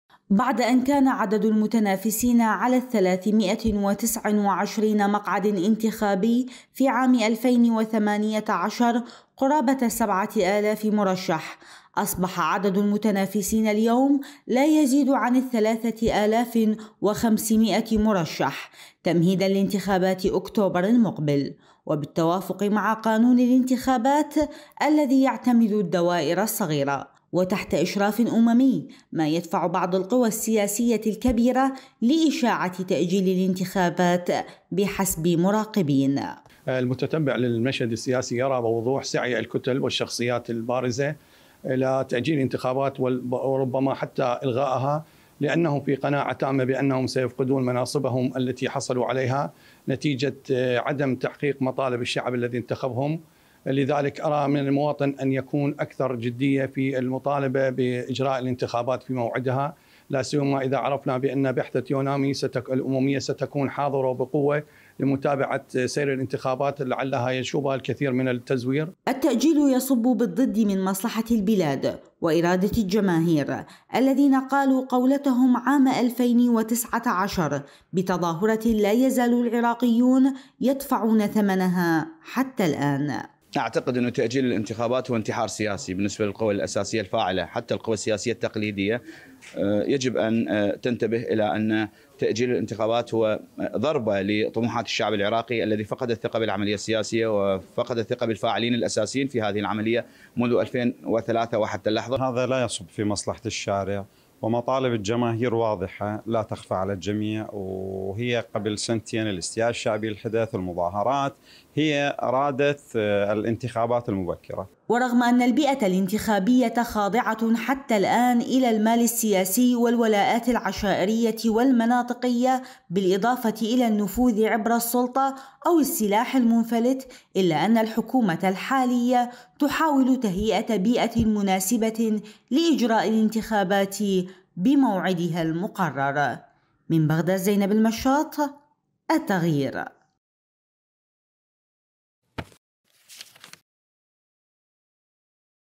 مساع سياسية لإيقاف الانتخابات البرلمانية | تقرير
مساع سياسية لتأجيل الانتخابات البرلمانية وسط حراك آخر لإجرائها بموعدها
تقرير – زينب المشاط